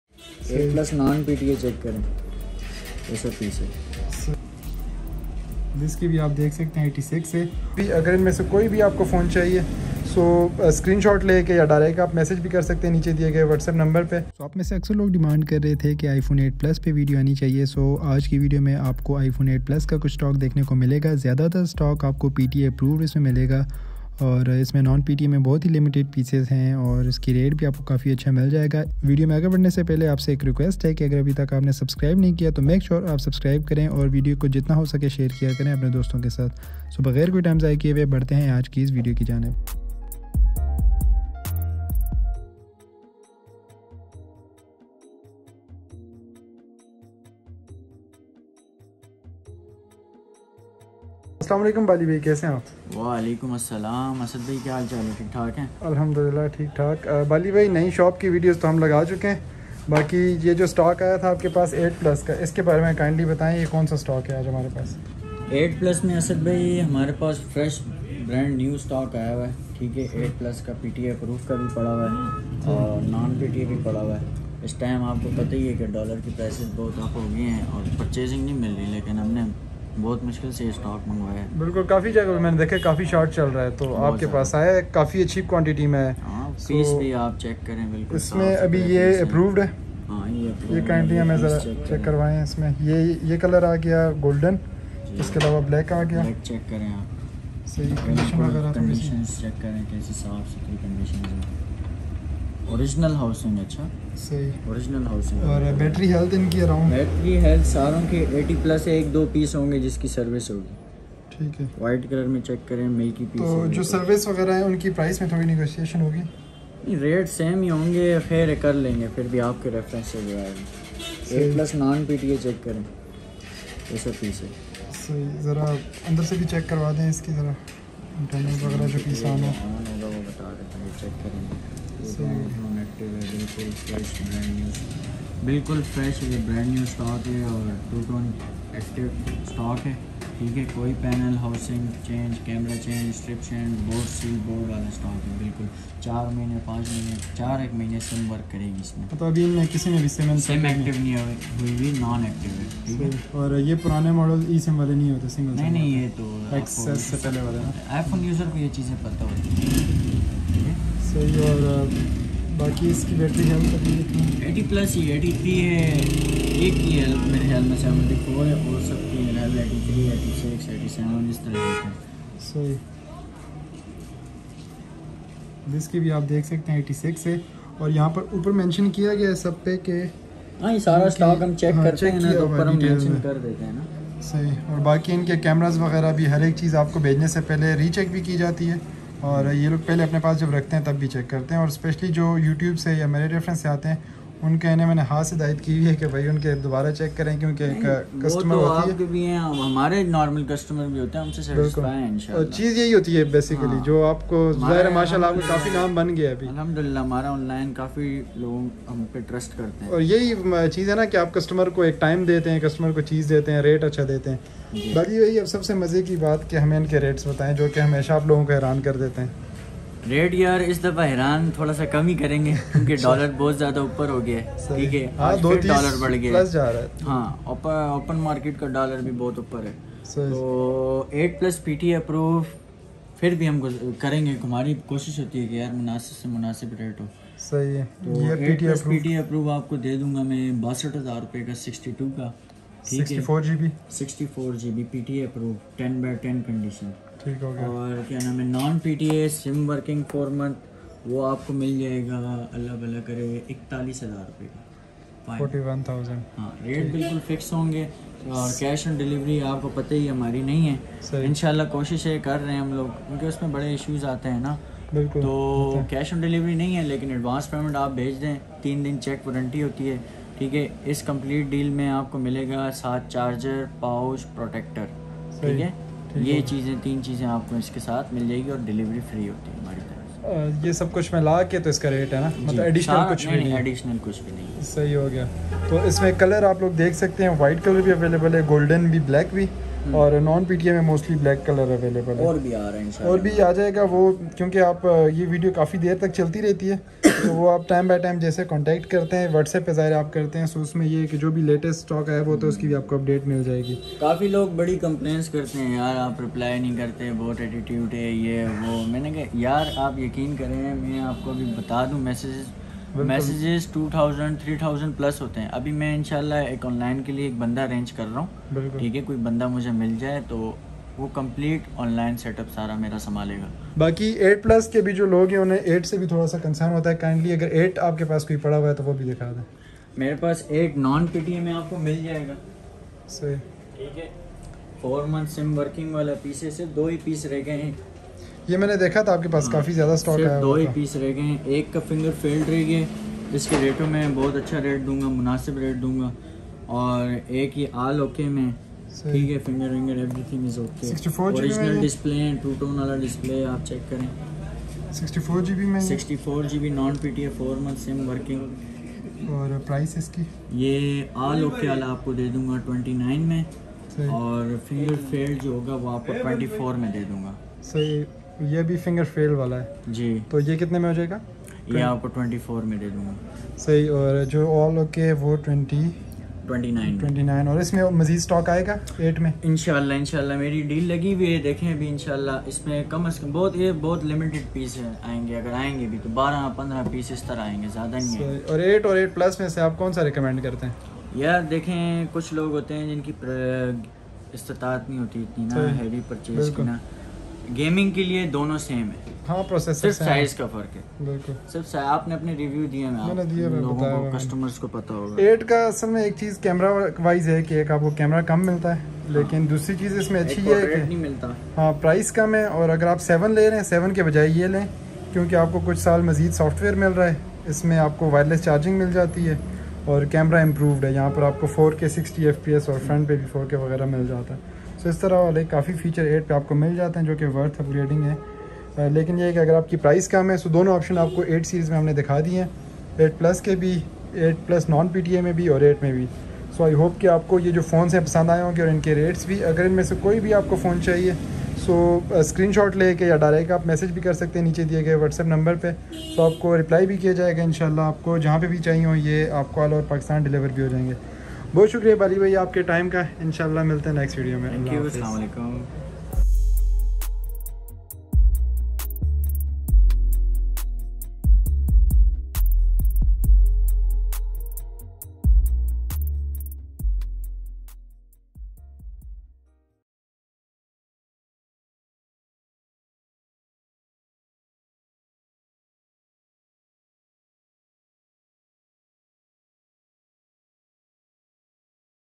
iPhone 8 Plus Non PTA & Approved Stock 2022 - Used & Brand New iPhones Prices in Pakistan 2022
AOA Everyone. In this video I've Apple#Man in Mansehra for prices of all used and brand new stock of iPhones So if you are interested in any device shown in this video feel free to contact them.

Call / Text / WhatsApp on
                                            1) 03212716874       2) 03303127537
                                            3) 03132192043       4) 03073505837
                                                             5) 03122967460